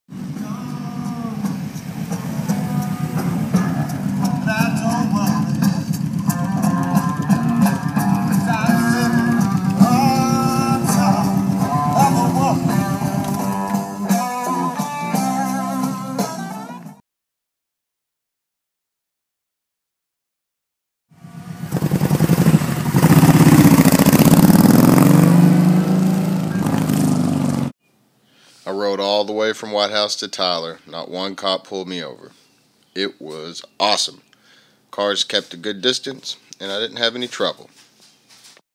Open carry AK47 TX
No mag in the gun, though I did have five 30 round mags with me.